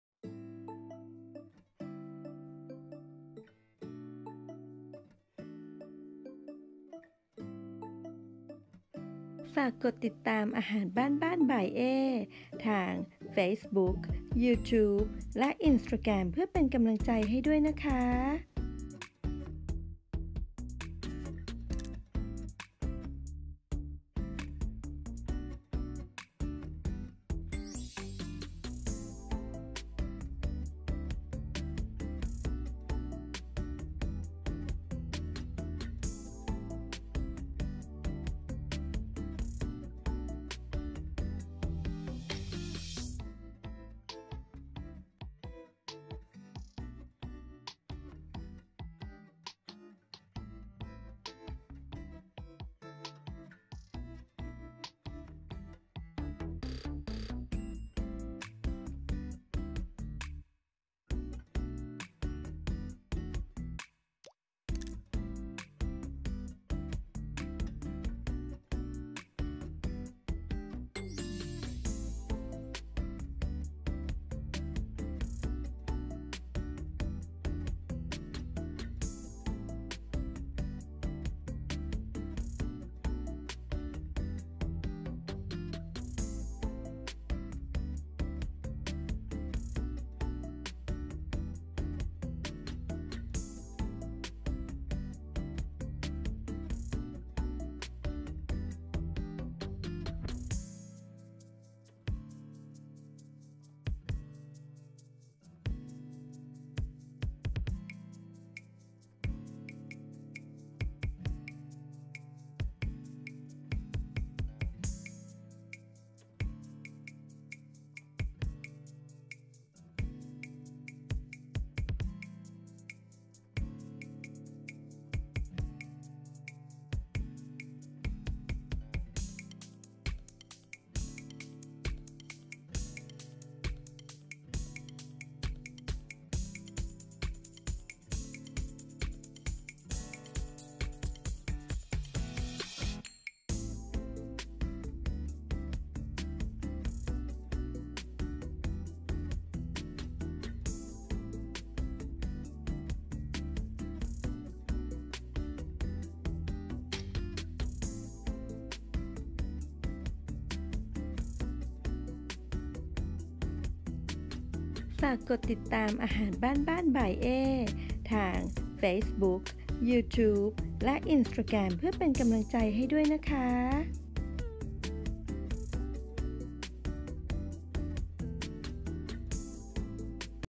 วิธีทำไข่ดาวจิ๋ว ไข่ครกจิ๋ว เตาเค้กป๊อป ใช้ไข่ไก่แทนไข่นกกระทา
เตรียม
ซอสปรุงรส
ถุงพลาสติก

วิธีทำ ดูในคลิปน๊า